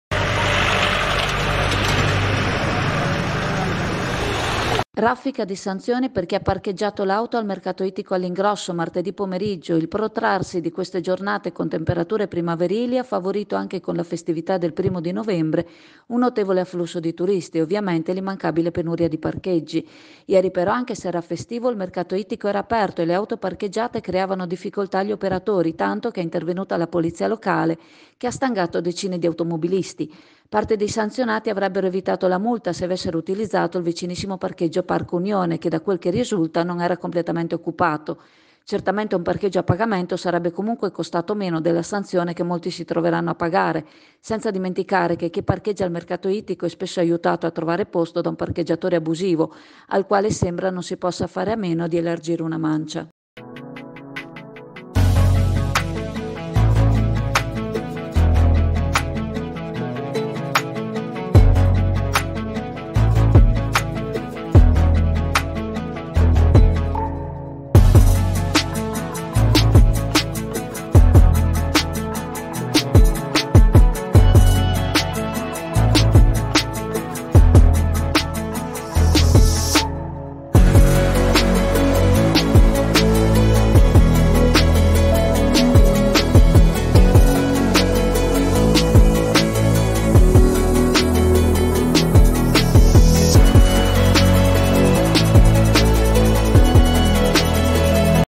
MERCATO ITTICO APERTO STANGATA DI MULTE AI TURISTI
Raffica di sanzioni per chi ha parcheggiato l’auto al mercato ittico all’ingrosso martedì pomeriggio. Il protrarsi di queste giornate con temperature primaverile ha favorito anche con la festività del primo di novembre un notevole afflusso di turisti e, ovviamente, l’immancabile penuria di parcheggi.
Ieri però anche se era festivo il mercato ittico era aperto e le auto parcheggiate creavano difficoltà agli operatori tanto che è intervenuta la polizia locale che ha “stangato” decine di automobilisti.
Parte dei sanzionati avrebbero evitato la multa se avessero utilizzato il vicinissimo  parcheggio Park Unione che, da quel che risulta, non era completamente occupato.
Certamente un parcheggio a pagamento sarebbe costato meno della sanzione che molti si troveranno a pagare, senza dimenticare che chi parcheggia al mercato ittico è spesso aiutato a trovare posto da un parcheggiatore abusivo al quale sembra non si possa fare a meno di elargire una mancia.